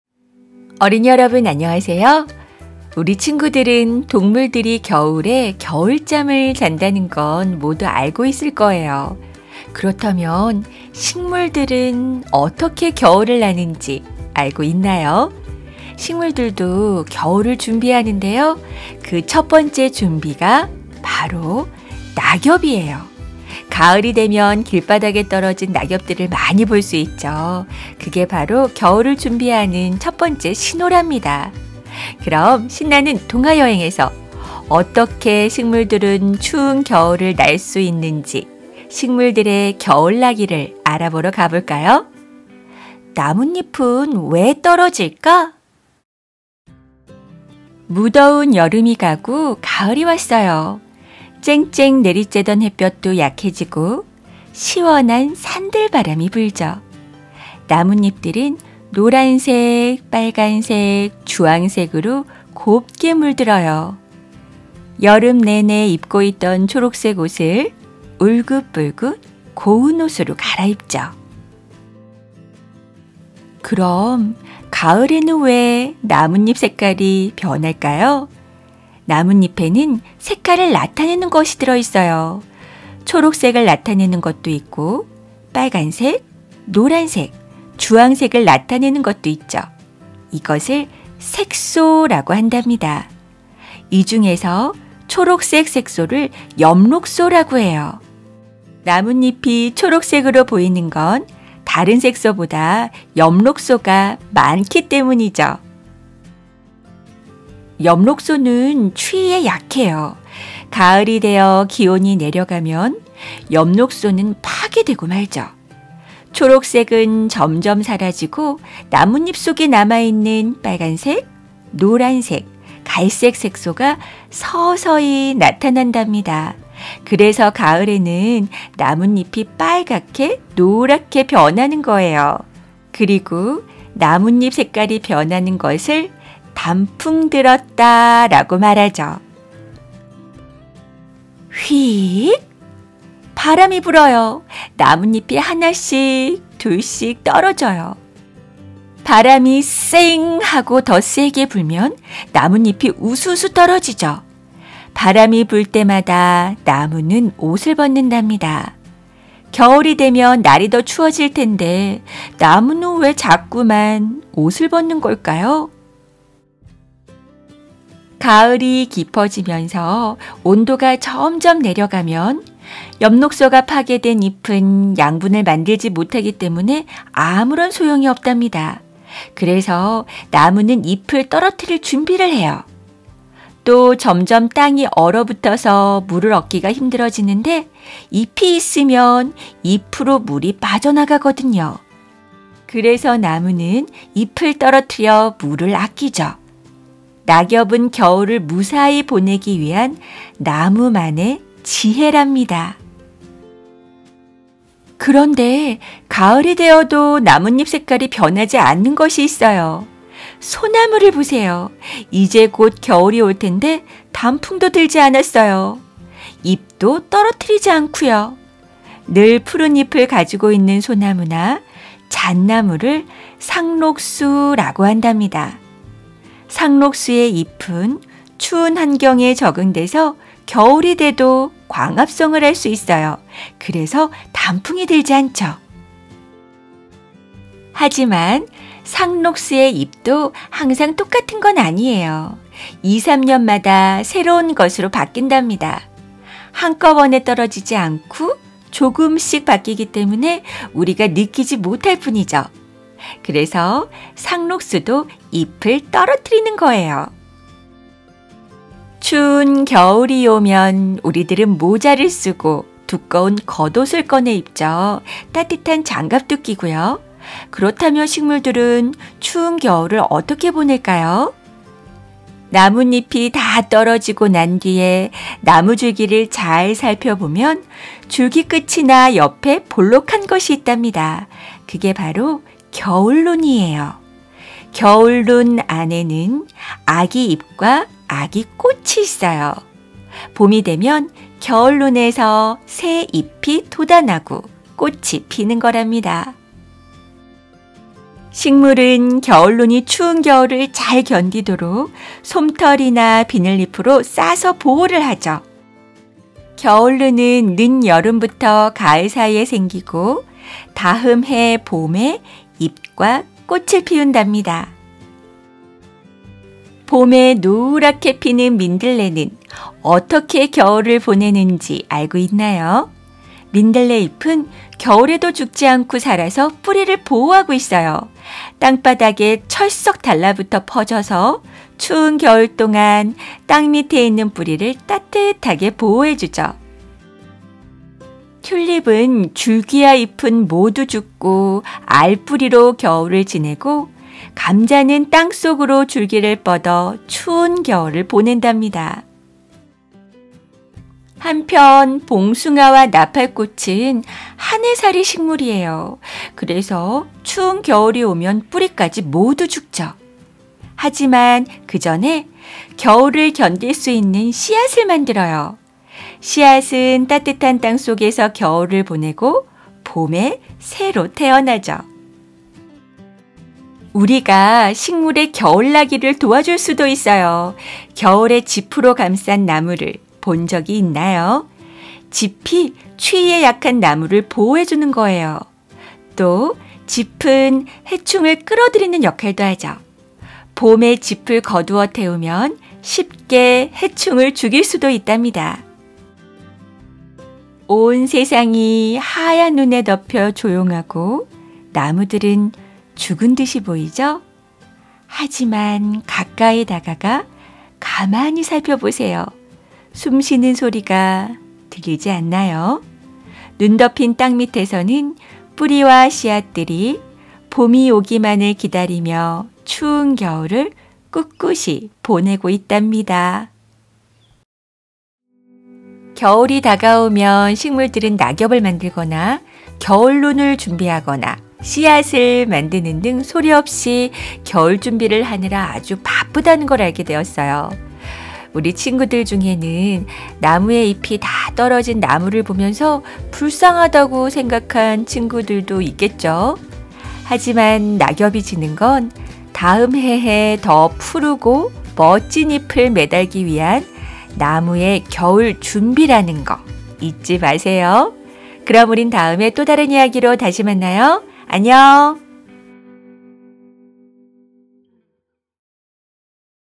[한글쌤의 동화책읽어주기]과학동화- 나뭇잎은 왜 떨어질까| 단풍 | 동화책| 지식동화 | 교육동화| 초등과학| 구연동화
어린이 여러분, 안녕하세요.
우리 친구들은 동물들이 겨울에 겨울잠을 잔다는건 모두 알고 있을 거예요.
그렇다면 식물들은 어떻게 겨울을 나는지 알고 있나요?
식물들도 겨울을 준비하는데요.
그 첫번째 준비가 바로 낙엽이에요.가을이 되면 길바닥에 떨어진 낙엽들을 많이 볼수 있죠.
그게 바로 겨울을 준비하는 신호랍니다.
그럼, 신나는 동화여행에서 어떻게 추운 겨울을 날수 있는지, 식물들의 겨울나기를 알아보러 가 볼까요?

도서정보
나뭇잎은 왜 떨어질까
글:박혜은
그림: 김진령
출판사: 한국노벨
본 동화는 더이상 판매되지 않는 절판된 도서입니다.

영상과 관련 궁금하신 부분이 있으시면 언제든 이메일로 부탁드려요.